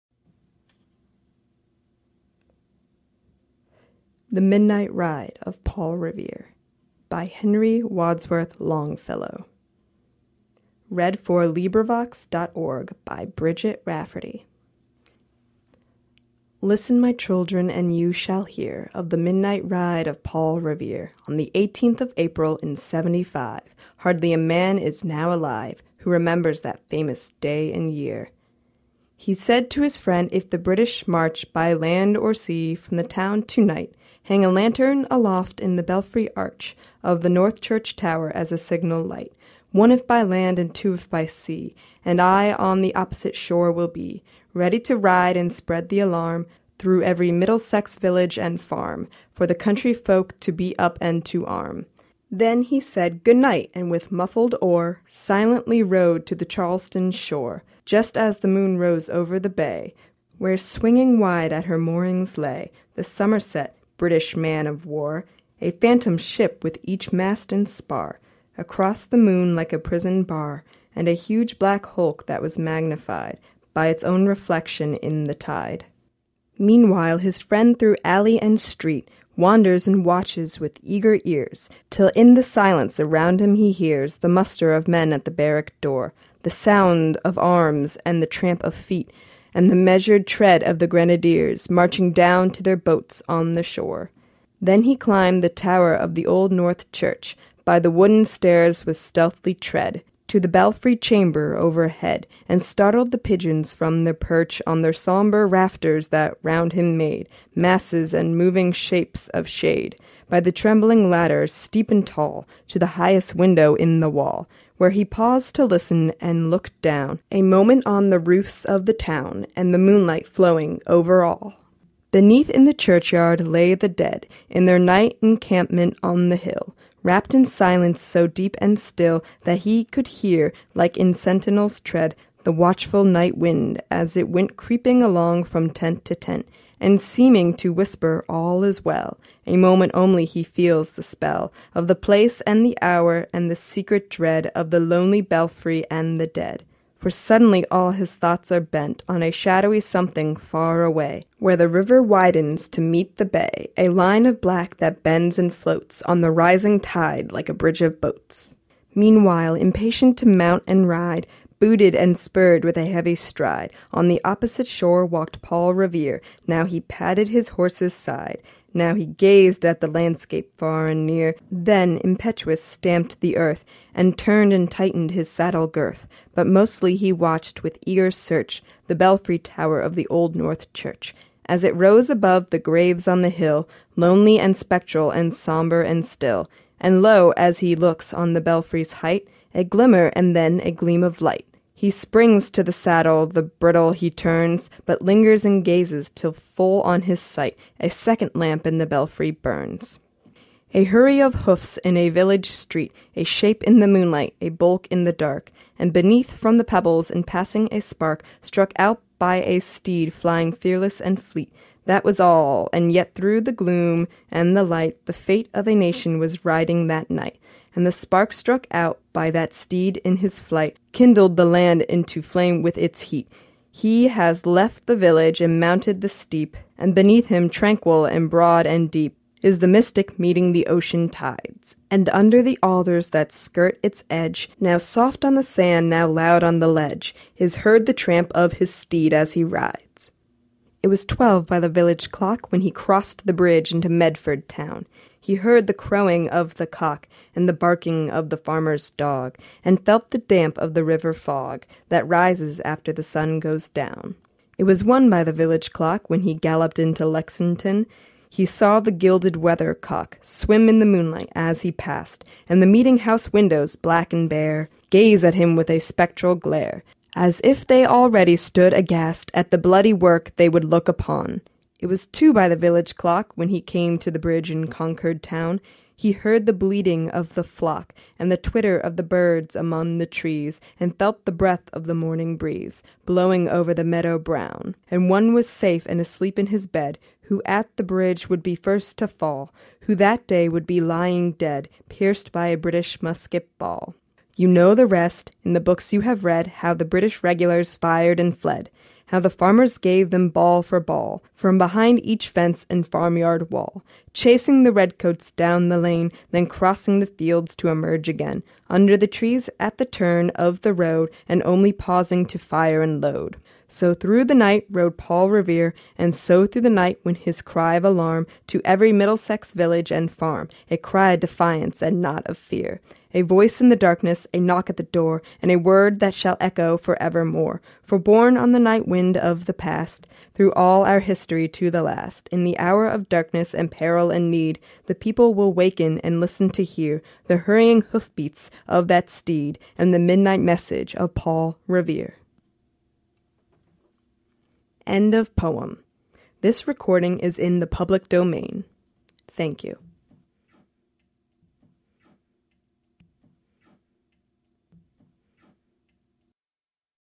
The Midnight Ride of Paul Revere: Ulysses by Henry Wadsworth Longfellow Black Screen For Sleeping
The Midnight Ride of Paul Revere: Ulysses (in  Long Poems Collection 006) by Henry Wadsworth Longfellow  Black Screen For Sleeping